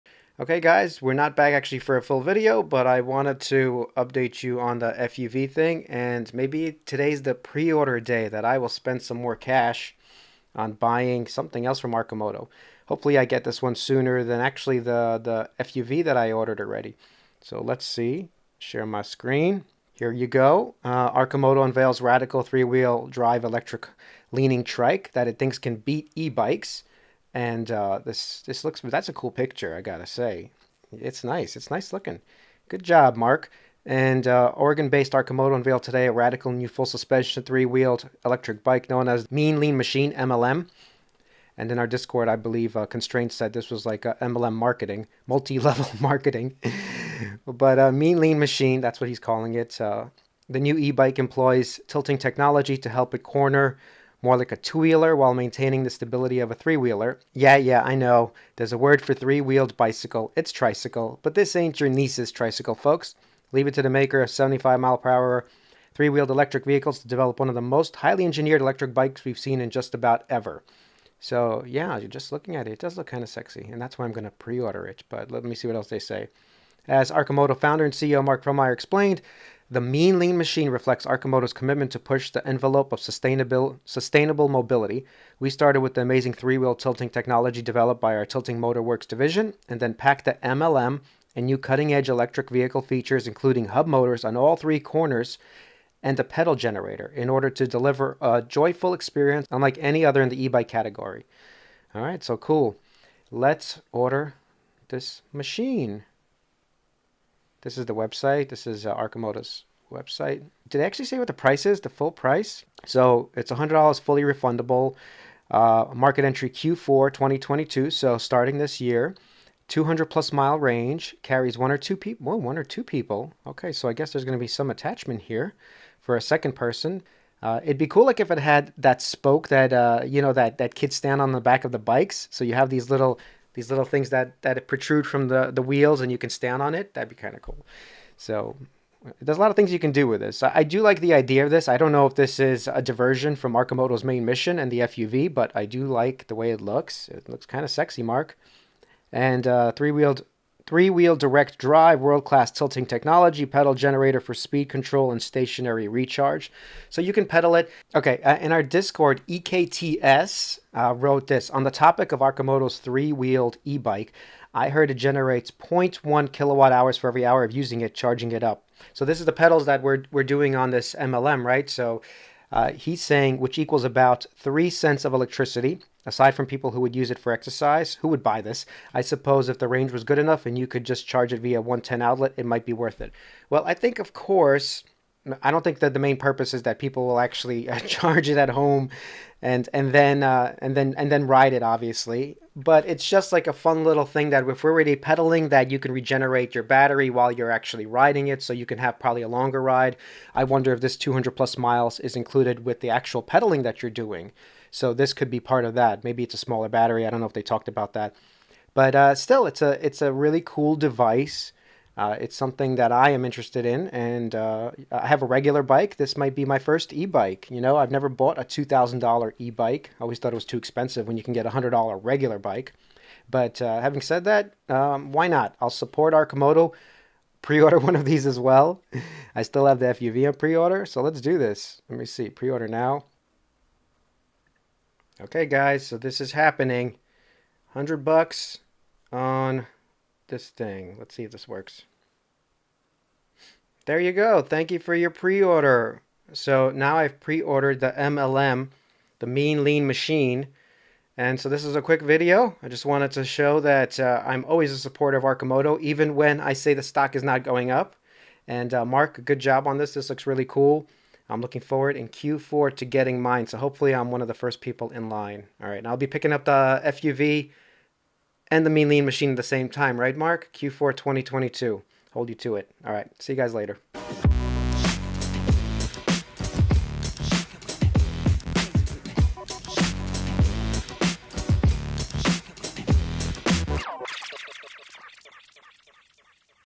I Just Preordered a Tricycle! Arcimoto's Mean Lean Machine E-Trike E-Bike
Another pre-order for me... My wife is going to disown me after this one.
I previously ordered an FUV for 20k - that was the first pre-order... and now the Lean Mean Machine E-Trike for 2k...

Thank goodness I'm an Arcimoto investor! :)

CHAPTERS

-The Freelancer Finance Fellas